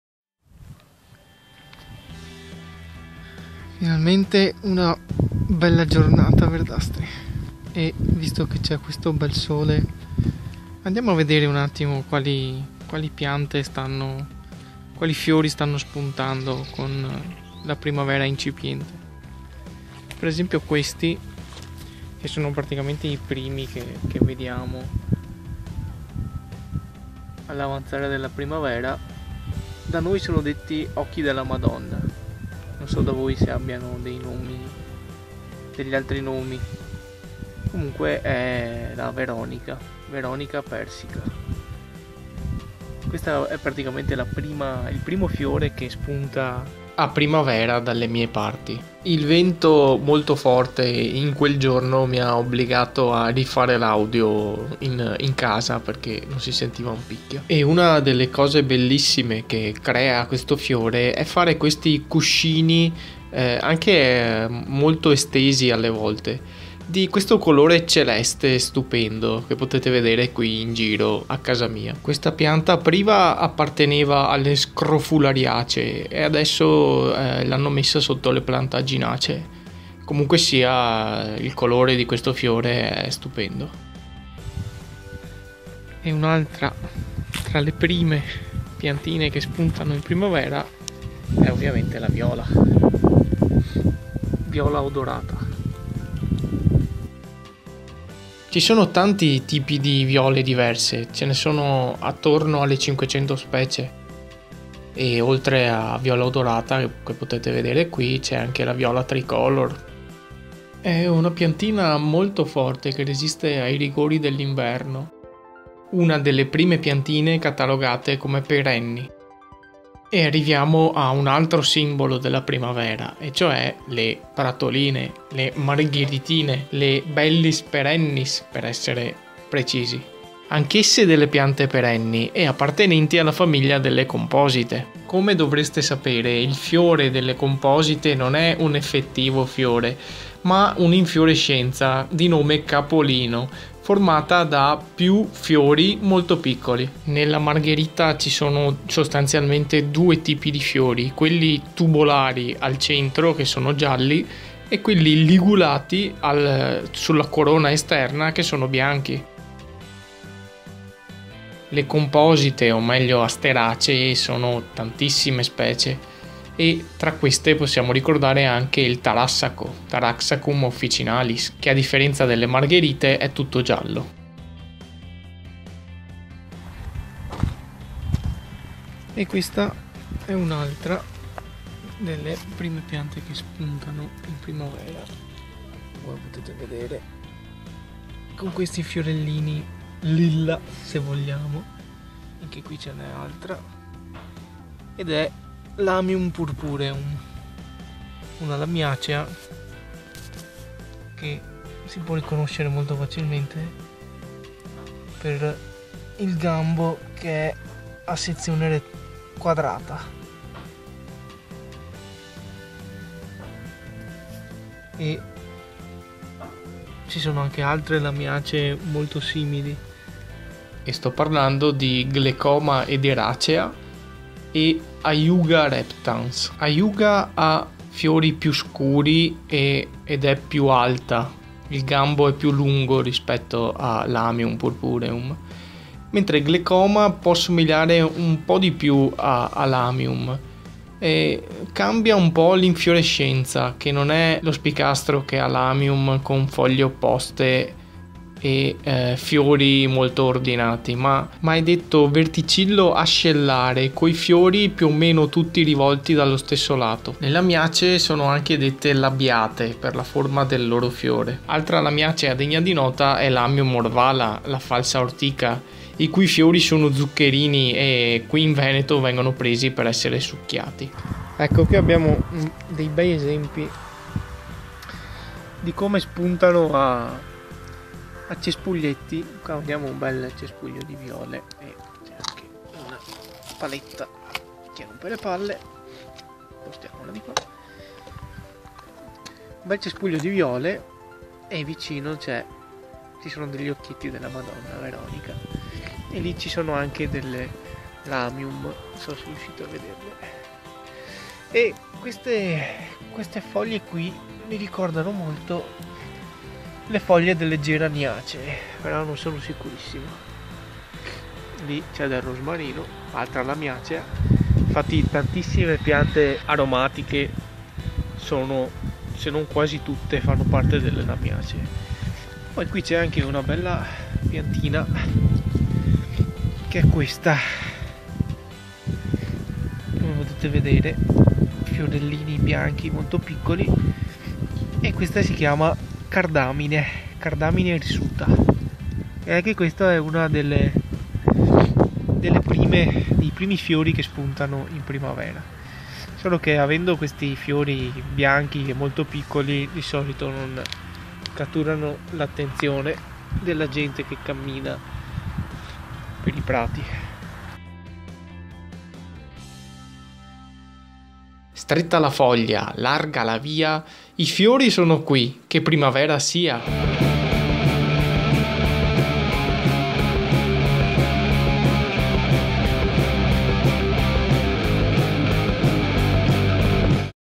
Primavera incipiente || La casa del Verde
La primavera è cominciata da poco e questo video fa riferimento a quando non era ancora cominciata per il calendario. Ma i segni del suo arrivo erano botanicamente innegabili.

Contatti